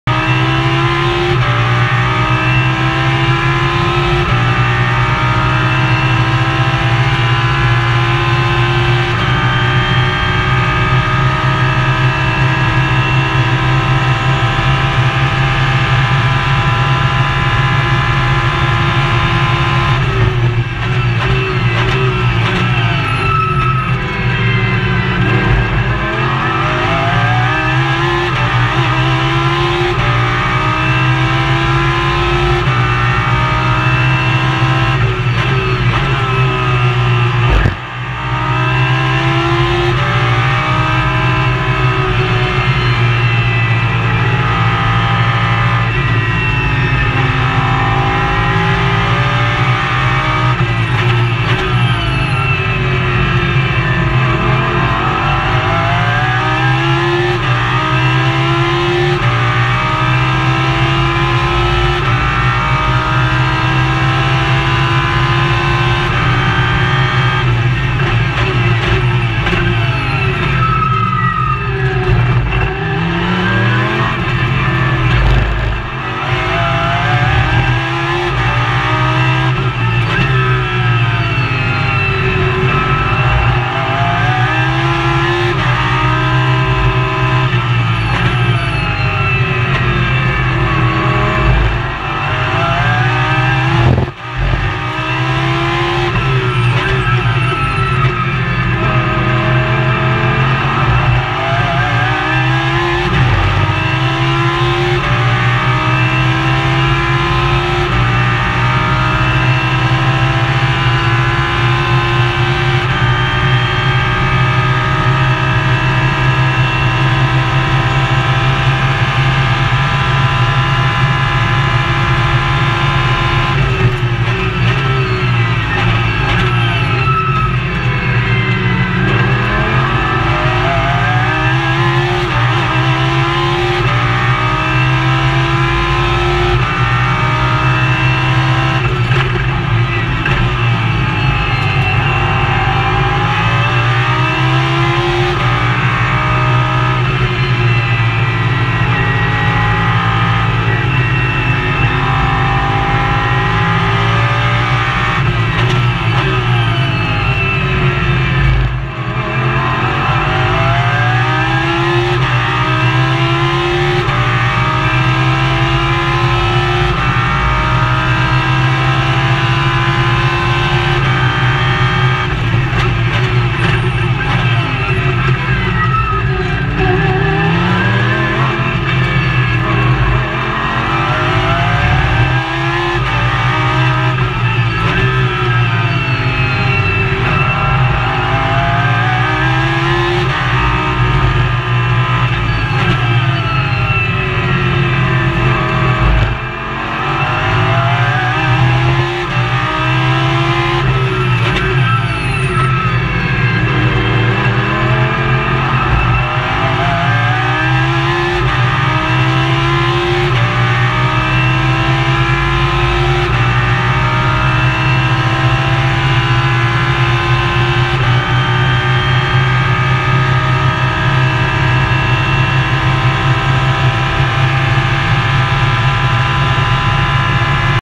Lamborghini Super Trofeo - Fuji
A few laps onboard the Janosz/Sowery Lamborghini around fuji mid-race as the rain started.